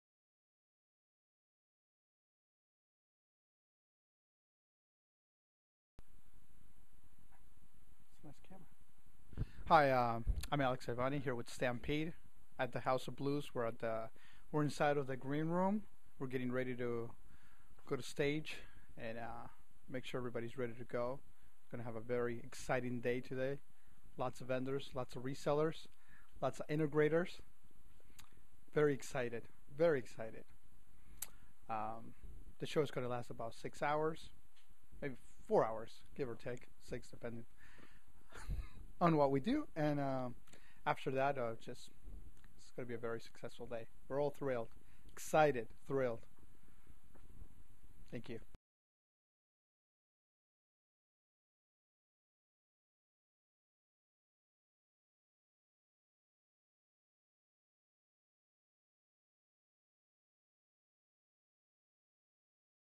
Big Book of AV Tour Kicks off in Boston
Stampede Product Manager Alex Ivani introduces you to the Big Book of AV Tour, Boston Event. Held at the House of Blues on August 11th. Watch for further videos in the this series to learn all about the Big Book of AV Tour and how to sign up for the next tour date in a city near you.

Stampede, the leading value-added distributor of presentation and home theater technology, kicked off their Big Book of AV tour in Boston Tuesday. The House of Blues in Boston came alive with the Big Book of AV tour August 11, 2010. New England area A/V dealers turned out as expected as Stampede presented its BIG BOOK of AV Tour at Boston's House of Blues. Coupled with an exciting and high-energy atmosphere, leading dealers and manufacturers gathered with for a day filled with product demonstrations, 3D education and new product announcements. Mike Williams, XpanD Cinema sales manager commented: "Our new partnership with Stampede came alive at the Boston BIG BOOK of AV Tour stop. It was clear to me that ProAV dealers were engaged and excited to learn about 3D applications for the commercial space. We are excited to partner with Stampede, the leading pro-AV distributor and their industry leading 3D initiatives." Jeff Blankensop from Magnetic 3D added: "We are confident that Stampede' s dealers are able to grasp new opportunities such as Magnetic 3D's displays which are perfect for digital signage applications as no glasses are needed. We are excited to partner with Stampede as they lead the way with their dealers in combining 3D opportunity with digital signage sales. This increases the revenue opportunity and ROI for dealers and advertisers. Stampede's ability to bring together so many dealers in specific territories around the country is a testament to their leadership role in our channels and with the dealers they serve. Nancy Onffroy, Regional Sales Manager with Samsung added: "Samsung continues to be impressed with the quality of the dealers that Stampede was able to attract to the BIG BOOK of AV Tour and the quality of the relationship that Stampede has with them. This is a tremendous benefit for Samsung as we work to leverage that relationship to sell more Samsung products." Stampede's entire Northeast field sales team attended the event and had the opportunity to host proAV dealers as they learned from more than 40 manufacturer exhibitor attendees. · Dealers attended training sessions from Sharp, XpanD Cinema, Insight Media and Epson · Gary Kayye, rAVEe Publications founder gave the keynote address and talked about the role of emerging technologies in the ProAV market and the opportunity for proAV dealers to capitalize on those new technologies · Over 40 manufacturer attendees exhibited throughout the House of Blues venue, which provided an exciting backdrop to a well attended event The Boston show was the first of 5 shows in 5 months for Stampede as the proAv distributor and its manufacturer partners bring the same high energy and experiences to House of Blues locations in each of Stampede's main US sales territories. The next shows are in Chicago, (September 14th), Orlando, Dallas and Los Angeles.